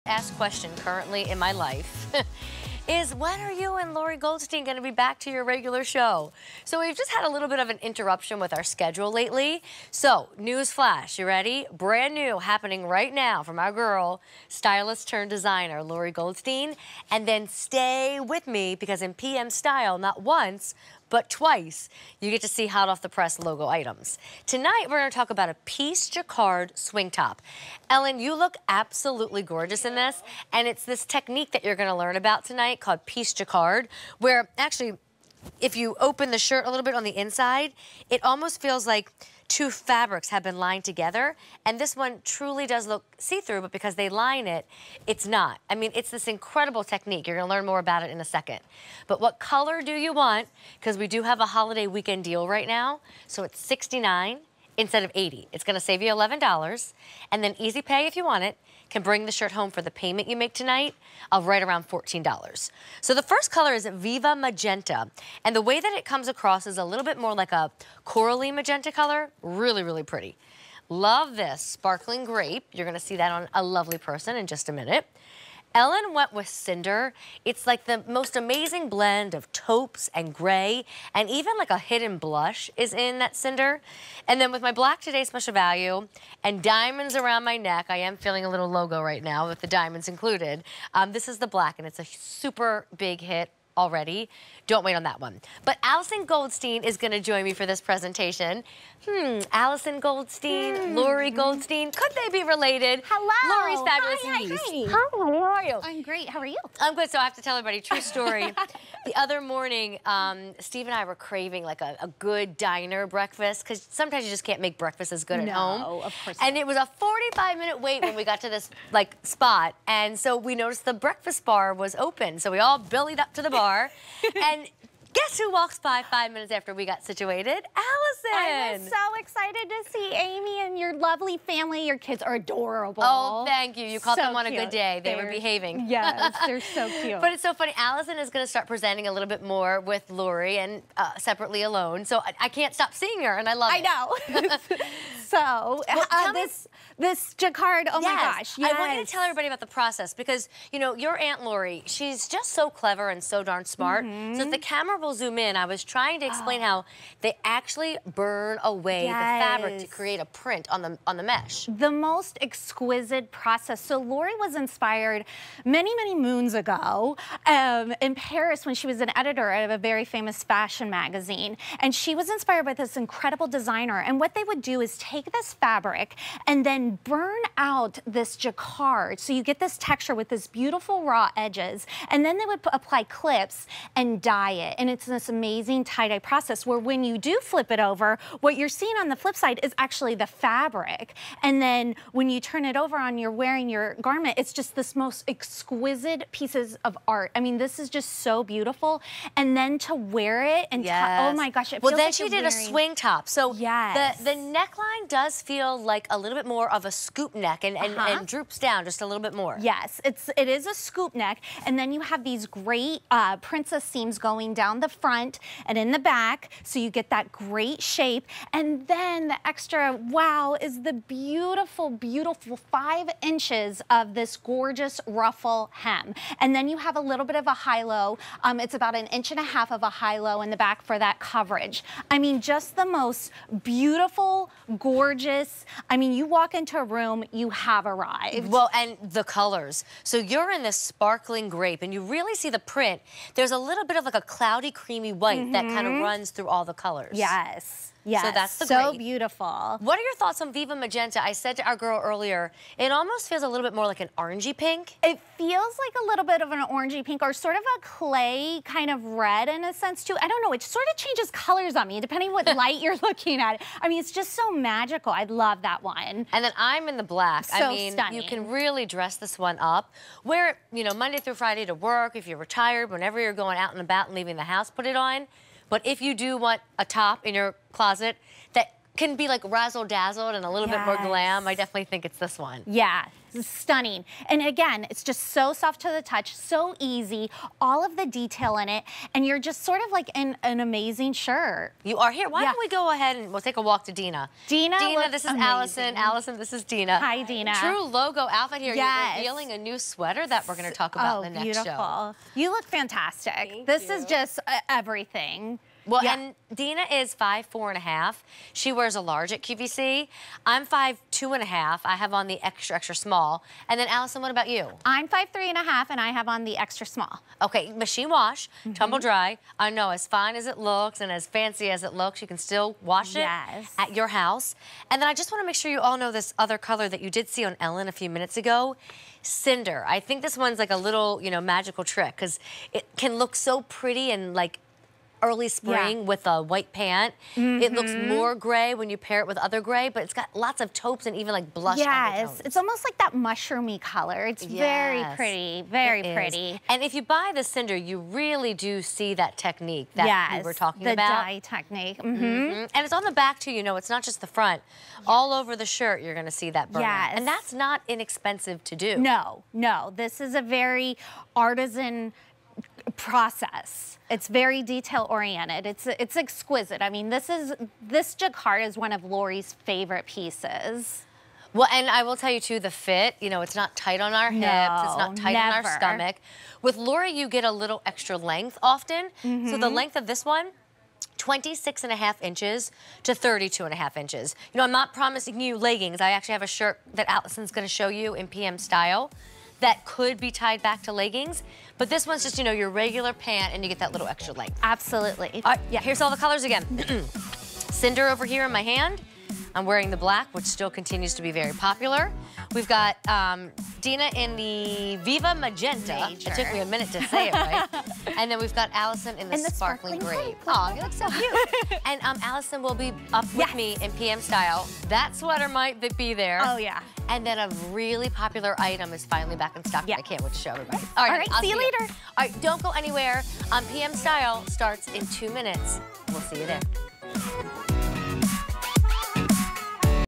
LOGO by Lori Goldstein Pieced Jacquard Swing Top on QVC
LOGO by Lori Goldstein Pieced Jacquard Swing Top

Pair this pretty peplum top with jeans and a pair of ballet flats. Now you're ready for a day of sightseeing in the city, an afternoon brunch with the girls, or dinner and drinks at your local eatery. From LOGO by Lori Goldstein&reg;.